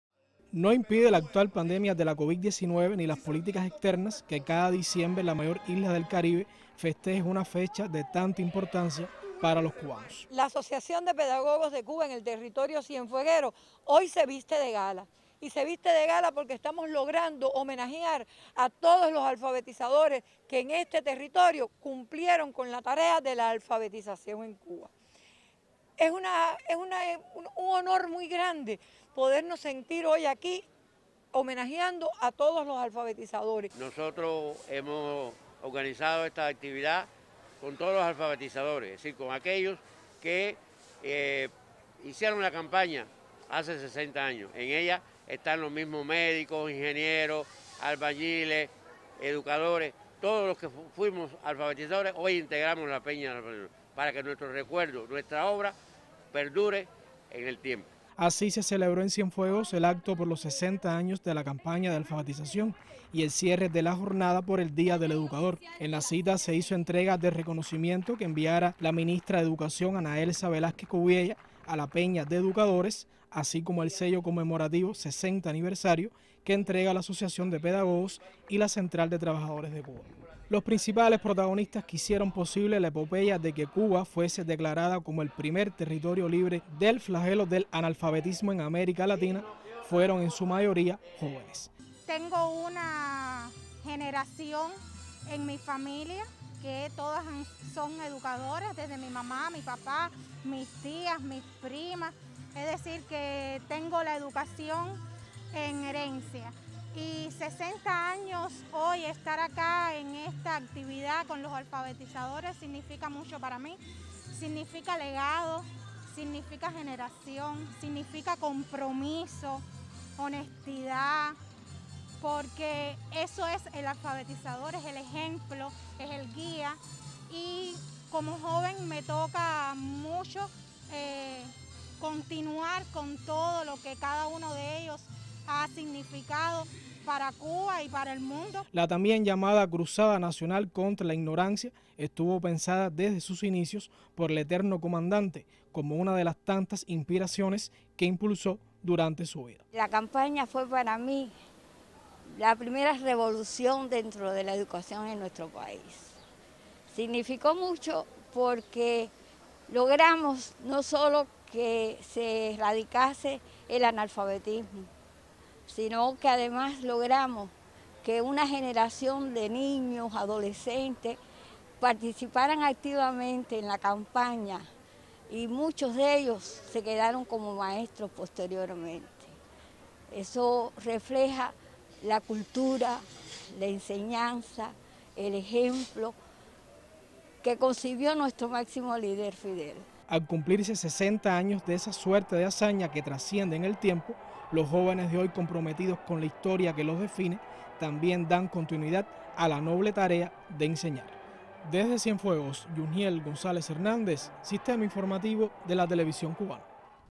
Culminan celebraciones por el aniversario 60 de la Campaña de Alfabetización en Cienfuegos
El acto por el cierre de las actividades dedicadas al aniversario 60 de la Campaña de Alfabetización y al Día del Educador, tuvo lugar en el centro recreativo Tropisur de Cienfuegos.